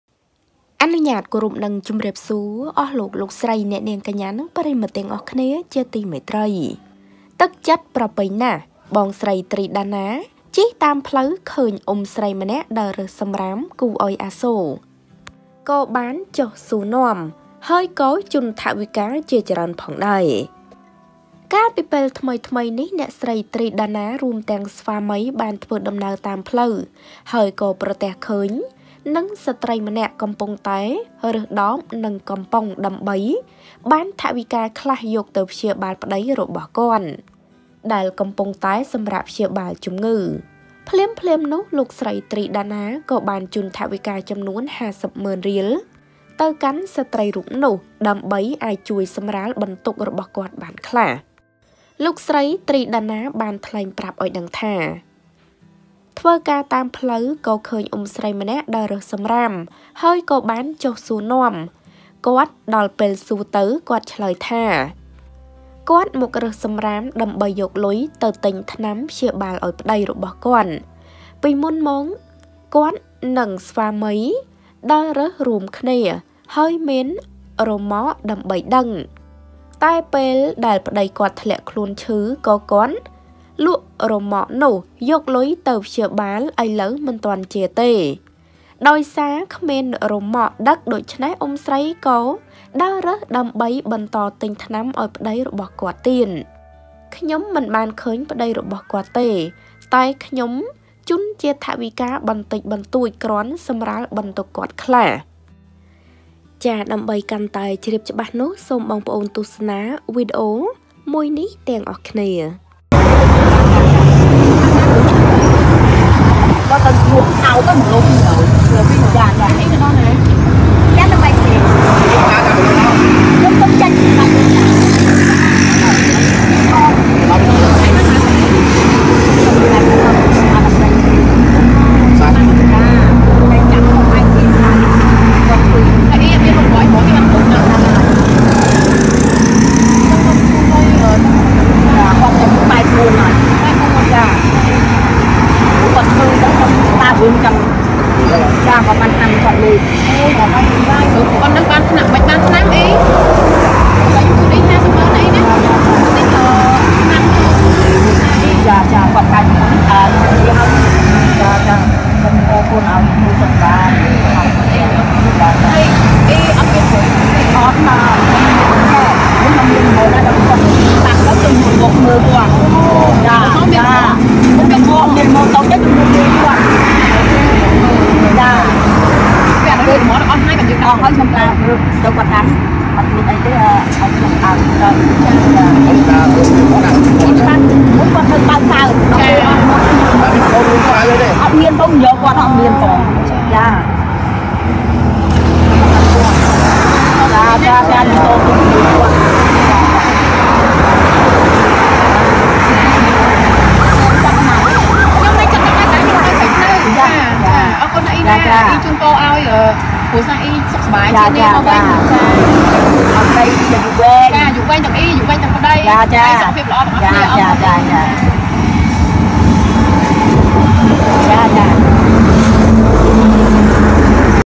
ទឹកចិត្តល្អក្រៃ!ទ្រី ដាណា ជិះតាមផ្លូវឃើញអ៊ុំស្រីម្នាក់ដើររើសសំរាមគួរអោយអាសូរ ក៏ចុះសួរនាំទើបដឹងថា
សូមអធ្យាស្រ័យ

ចំពោះការមិនបានតប ខំមិនបានគ្រប់គ្នា

Ms Socheata